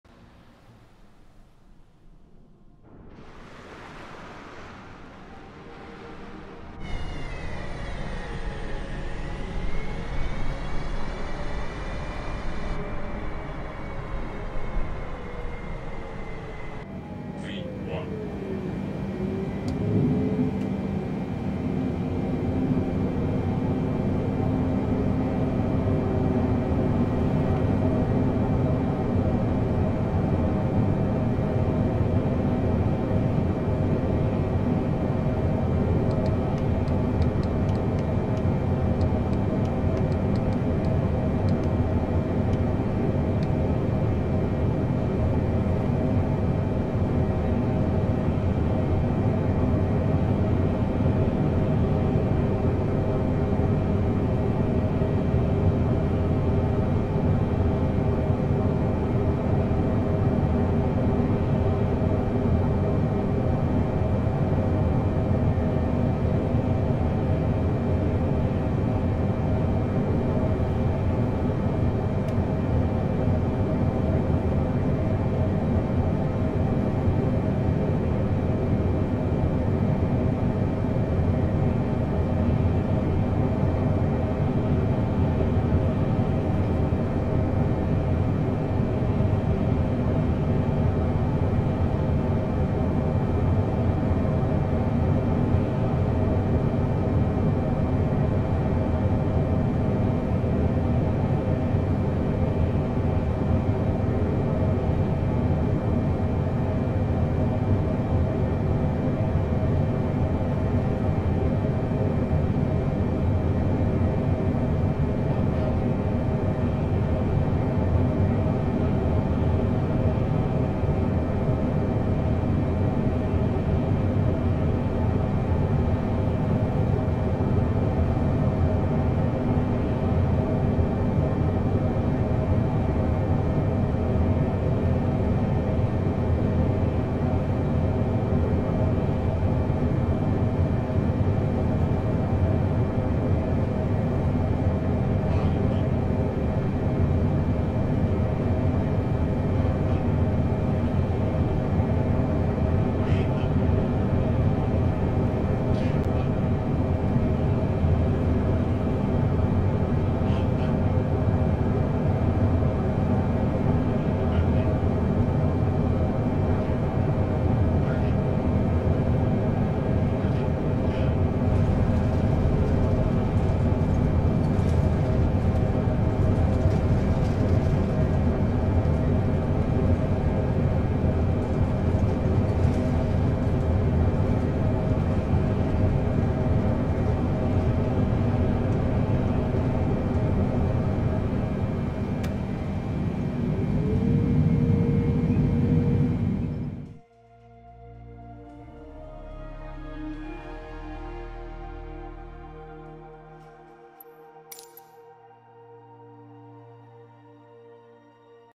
Landing B747-8i at John F Kennedy International Airport, (KJFK) New York
Arrival Airport: John F Kennedy International Airport, (KJFK) New York
Arrival Runway: 31L
Runway Elevation: 16 Ft AMSL

Weather condition: Clear Skies

Aircraft Type: Boeing 747-8 Intercontinental